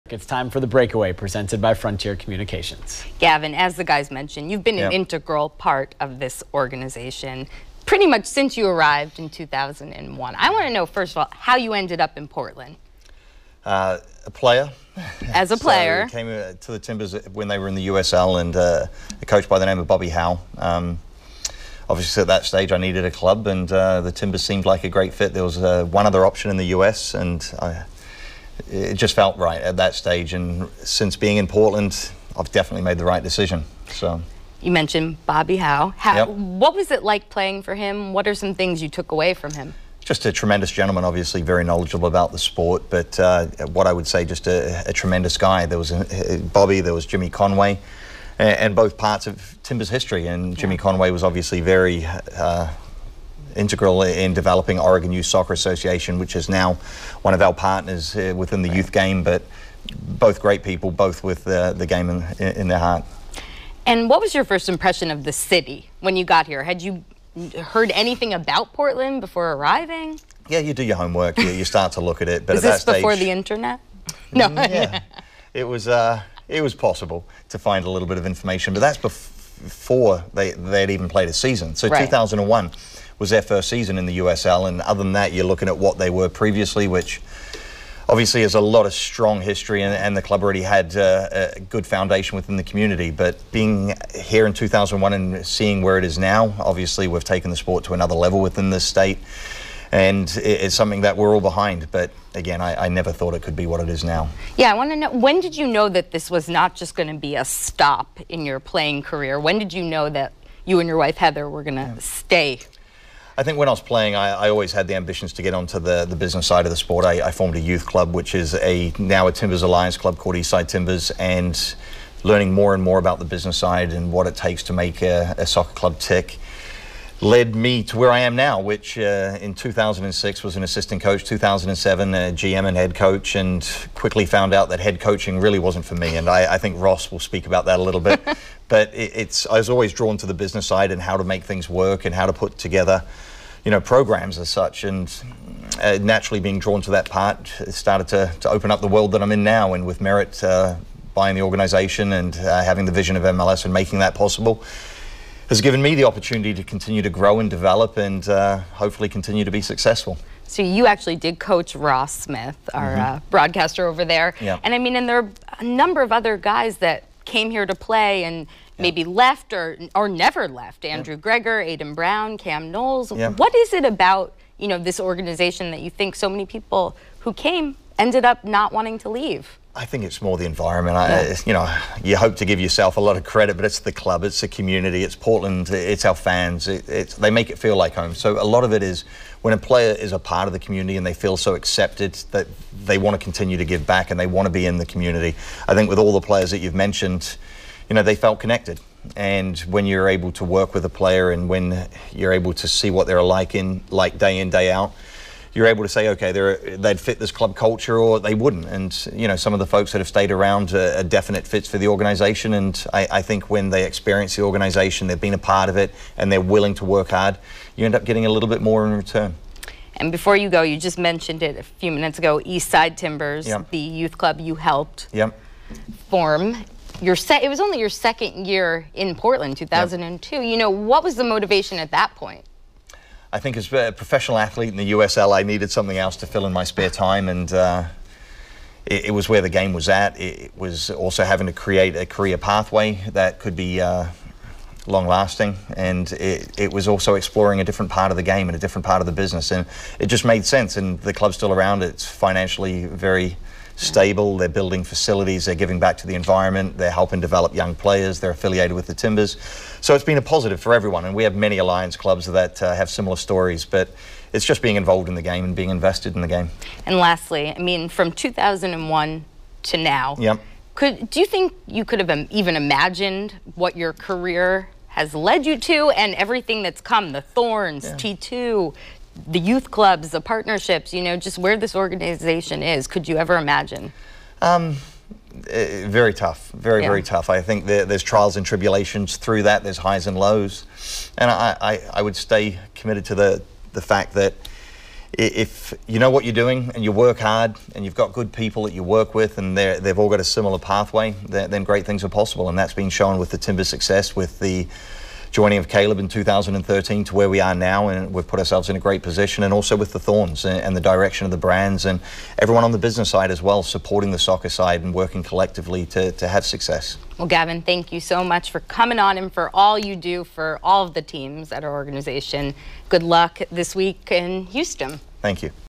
The Breakaway with Gavin Wilkinson | Timbers in 30 | Oct. 27, 2017
Timbers and Thorns GM/President of Soccer Gavin Wilkinson talks to Samantha Yarock about his early days in Portland and his path in the city from player to President of Soccer.